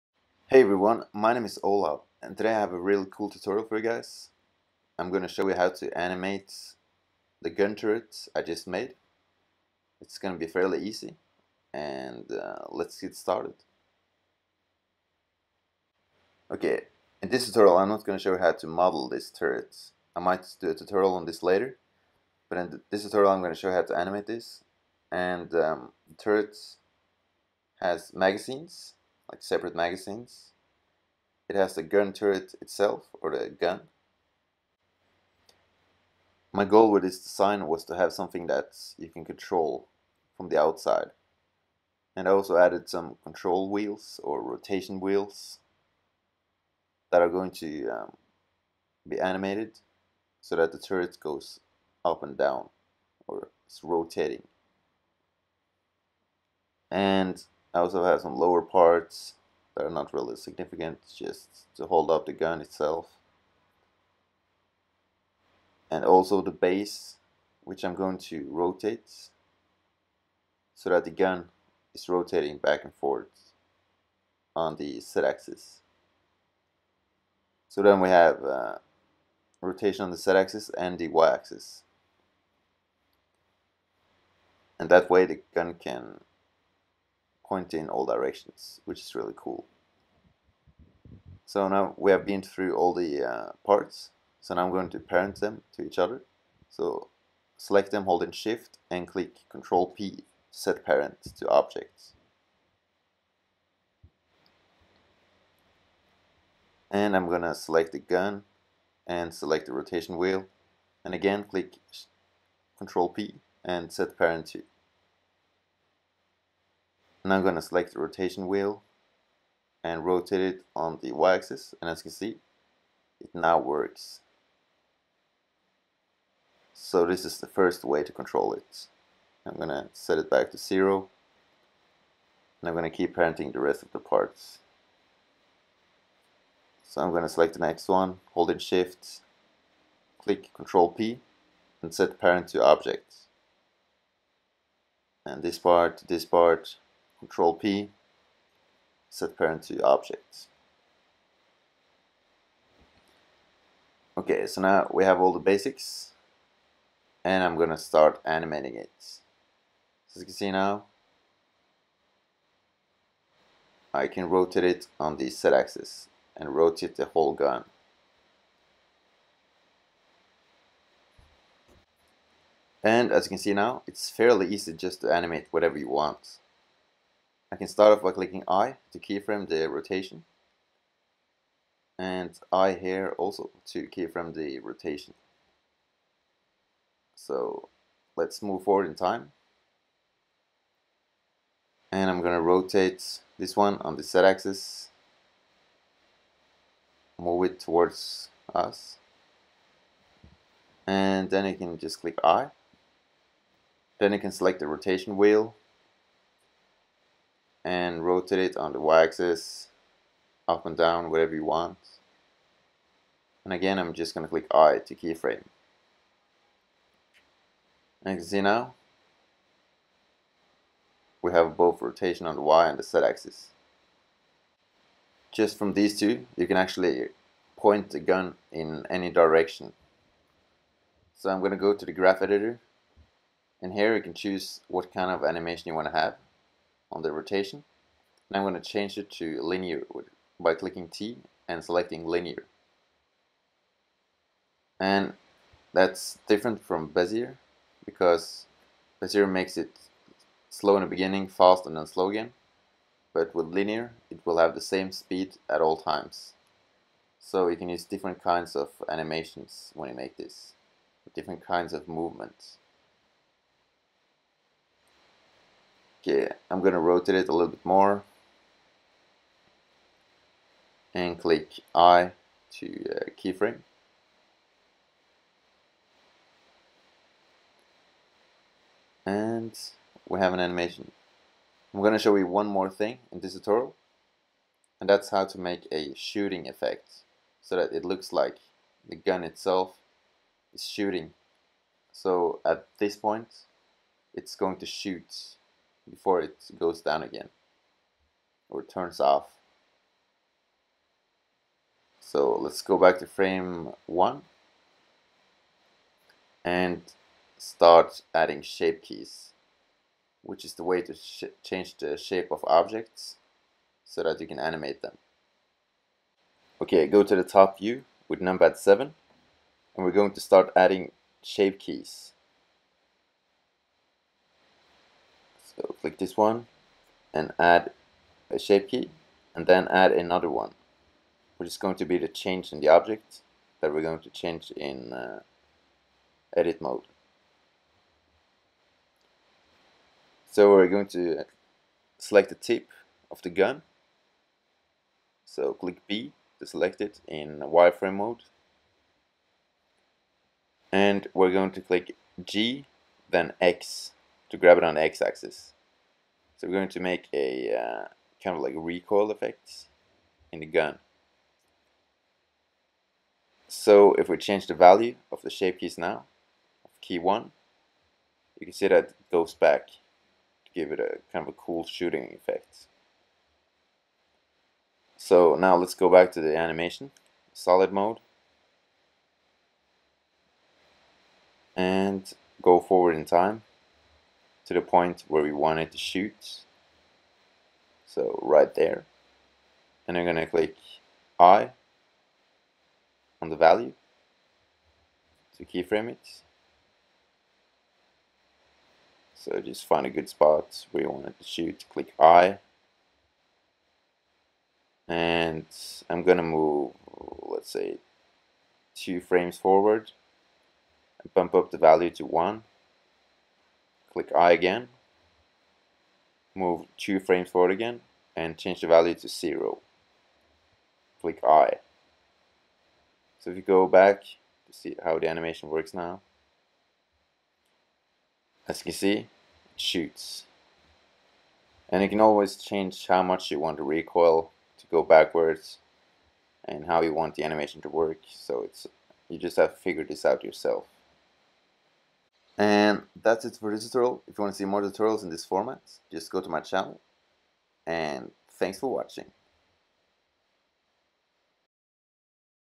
[2.79] Tutorial Robot Gun Turret Animation Blender
In this tutorial I will show you guys how to make a autonomous turret gun in Blender.